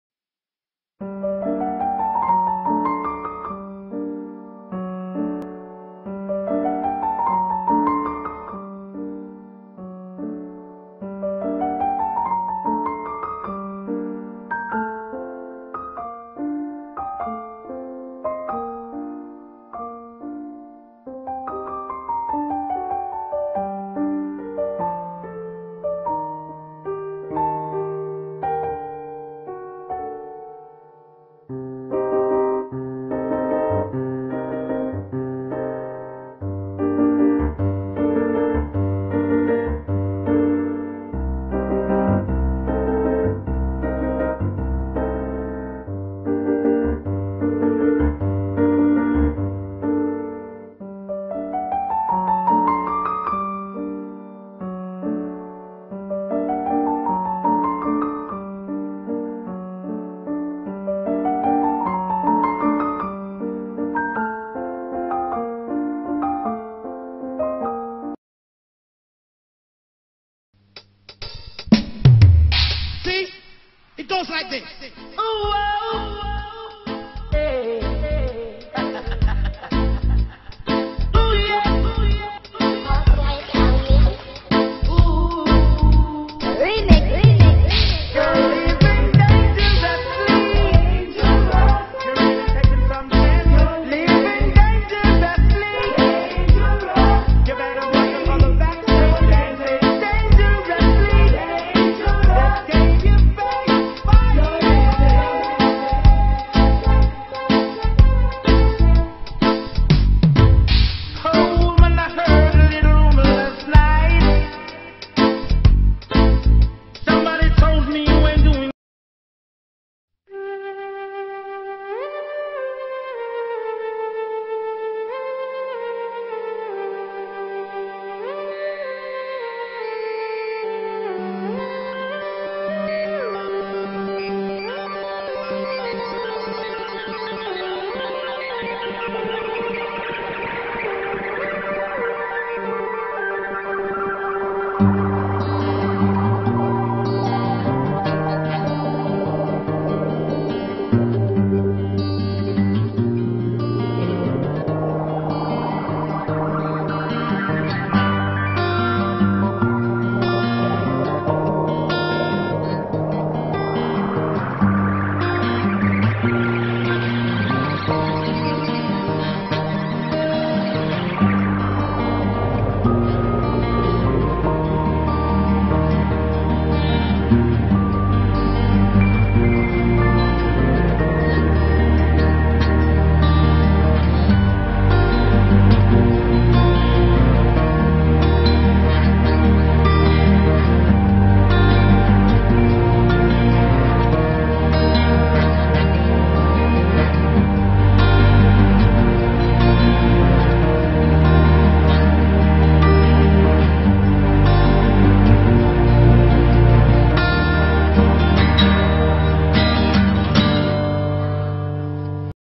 Cloud2 v4 realtime webGL sound visualization demo
capture of the realtime render of cloud2 a webGL sound visualization that I'm currently developing and which you can find

Now with controlers, presets, autofetch, autotraveler, etc :P